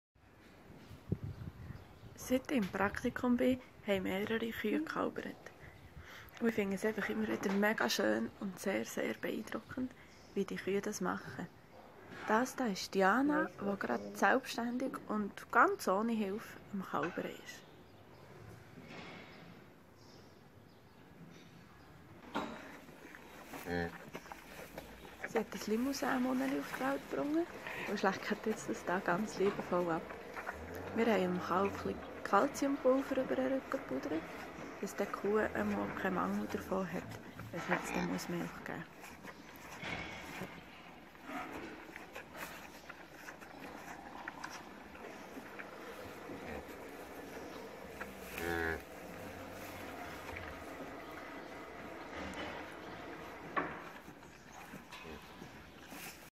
Bauernhof-Praktikum Teil 3 – Geburt eines Kalbes
Deborah Rentsch, Agronomie-Studentin an der ETH, macht diesen Sommer ein Praktikum auf dem Hof von Franziska Schärer in Melchnau. Hier zeigt Rentsch, wie spannend es jedes Mal wieder ist, wenn eine Kuh kalbt.